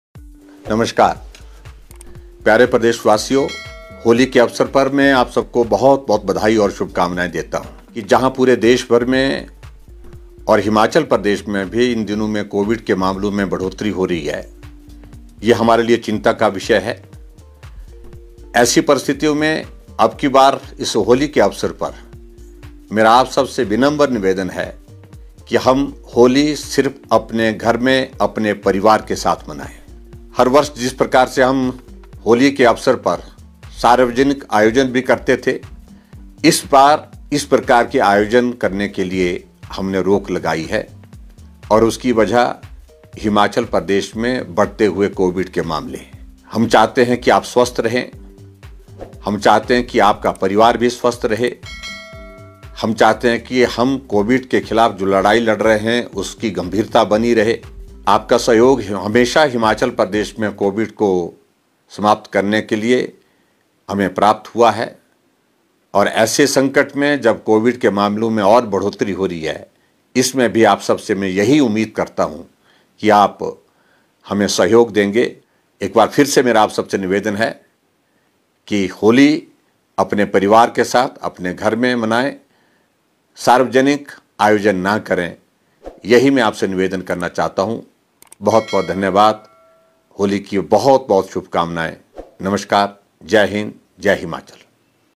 होली संदेश एवम् शुभकामनाएं : CM जयराम ठाकुर
Chief Minister Jairam Thakur congratulated the people of Holi